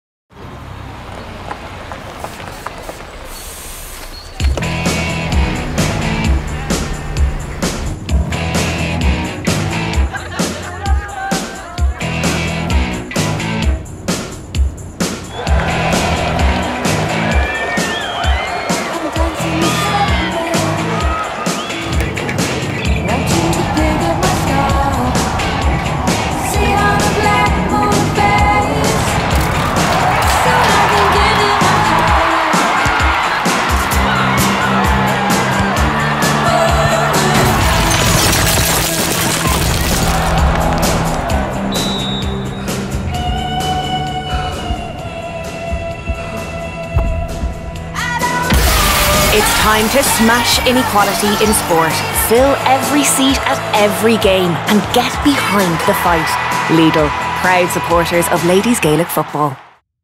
Get Behind The Fight for Ladies Gaelic Football | Lidl Ireland
It’s time to smash inequality in sport. Fill every seat at every game and get behind the fight. Lidl, proud supporters of Ladies Gaelic Football since 2016.